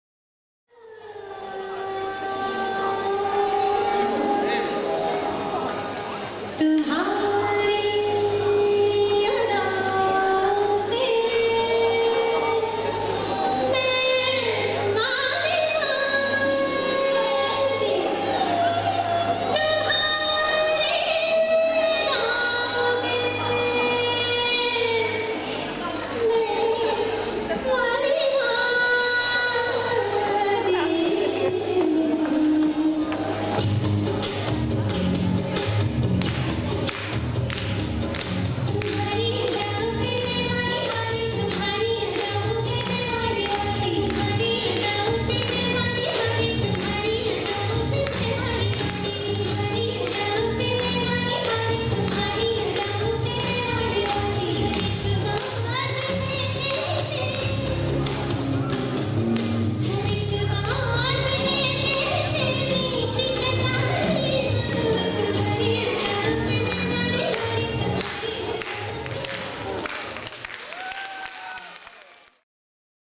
Irina Plodusheva ⊰⊱ V. Feeriya '12.
Ирина Плодушева на 6 фестивале "Восточная Феерия 2012".
21.04.12. Днепропетровск. Дворец Культуры Машиностроителей
(Молодежь+Взрослые Н Индийский танец) .